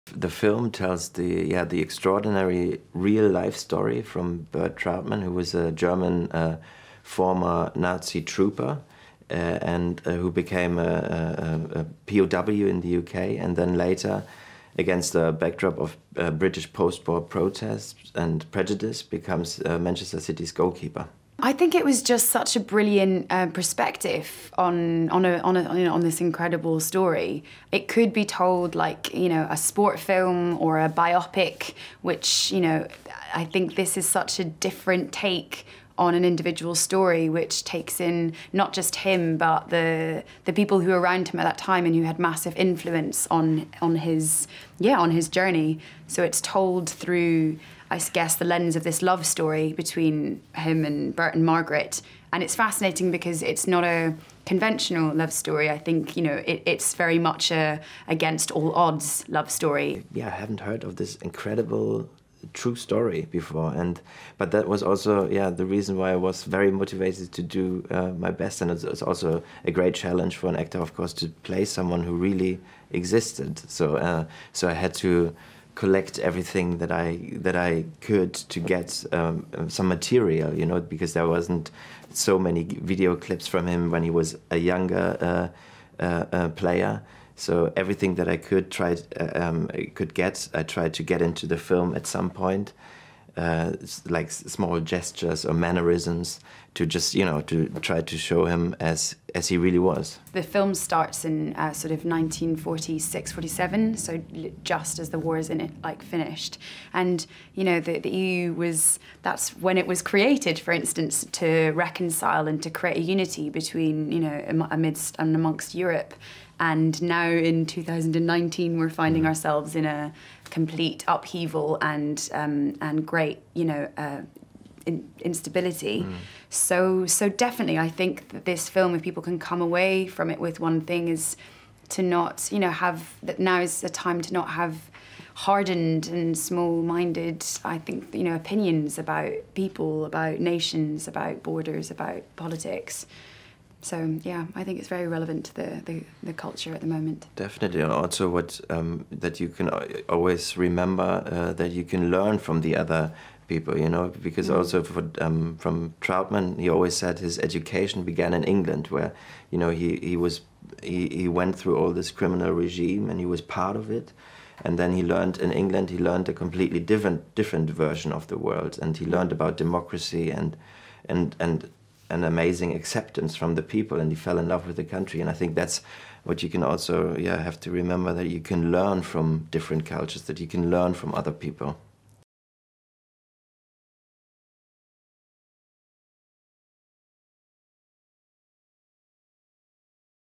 The Keeper - David Kross and Freya Mavor interview
The two leads of the story of Manchester City's greatest ever player talk about his remarkable story, and what it is that got them match-ready for the role.
The Keeper is in UK cinemas nationwide from Friday 5th April